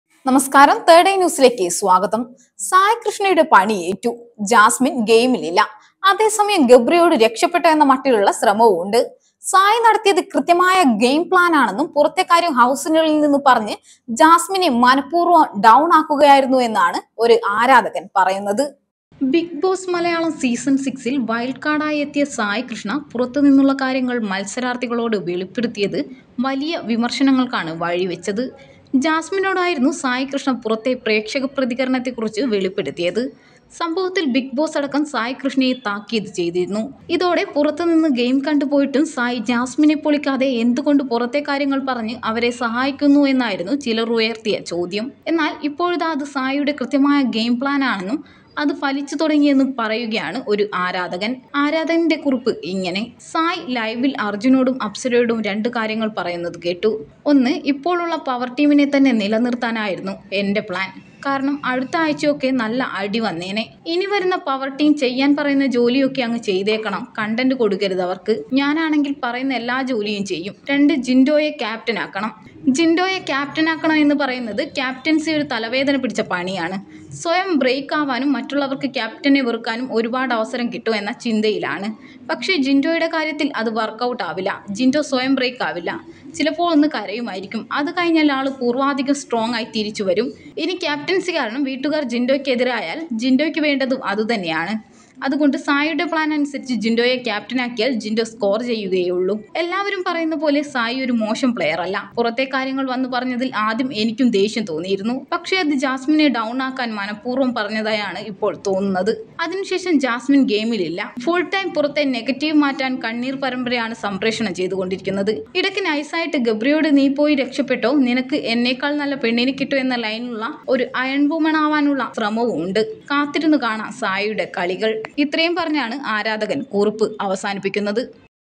സായിയുടെ പണി ഏറ്റു, ജാസ്മിൻ ഗെയിമിൽ ഇല്ല...ഗബ്രിയേൽ രക്ഷപ്പെട്ടോ എന്ന മട്ടിലുള്ള ശ്രമവും | Bigg Boss
സായിയുടെ പണി ഏറ്റു, ജാസ്മിൻ ഗെയിമിൽ ഇല്ല...ഗബ്രിയോട് രക്ഷപ്പെട്ടോ എന്ന മട്ടിലുള്ള ശ്രമവും! വൈയറലായി ആരാധകൻ്റെ കുറിപ്പ്